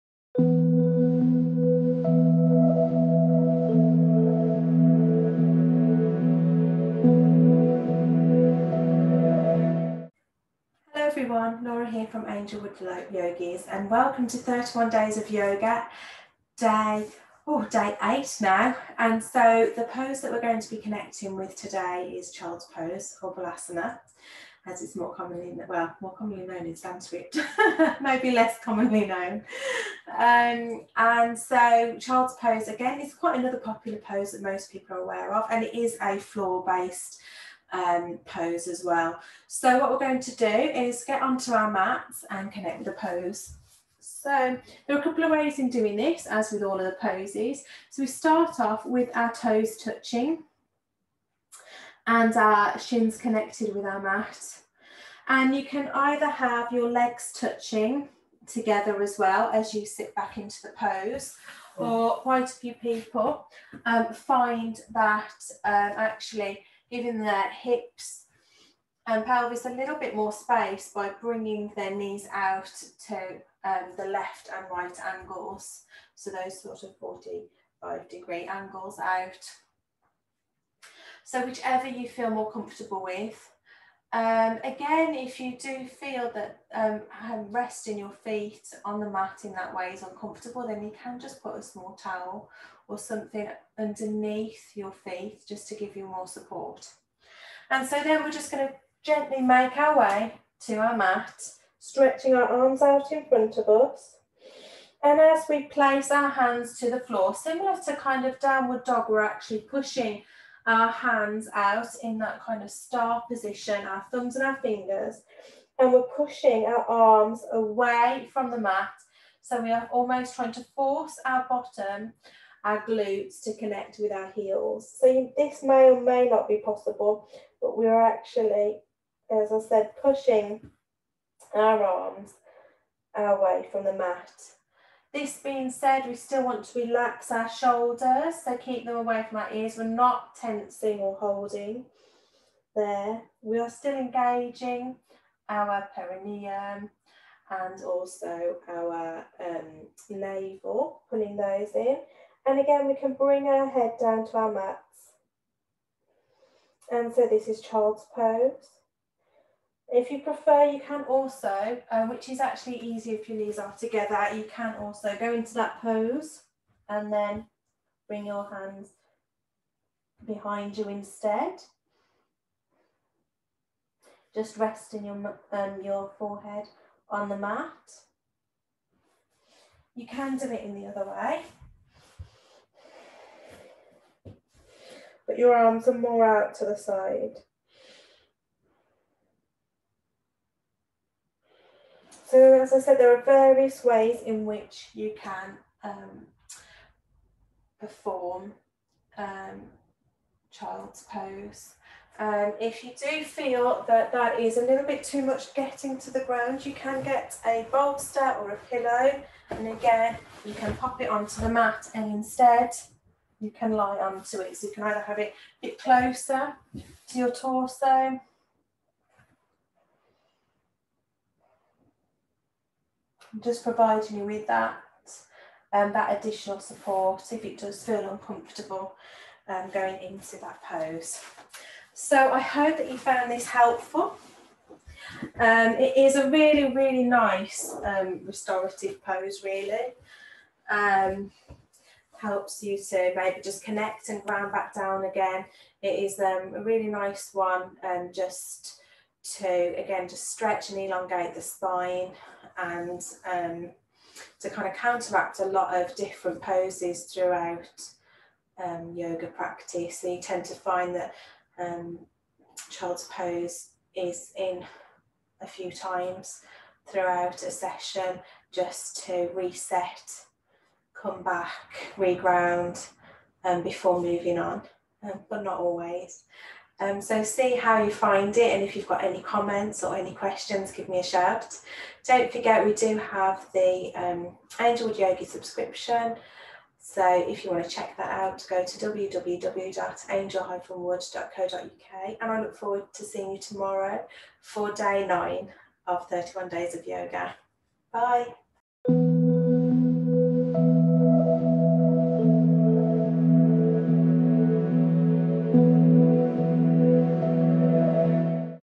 31 Days of Yoga - Day 8
Day of of 31 Days of Yoga takes us through Child's Pose.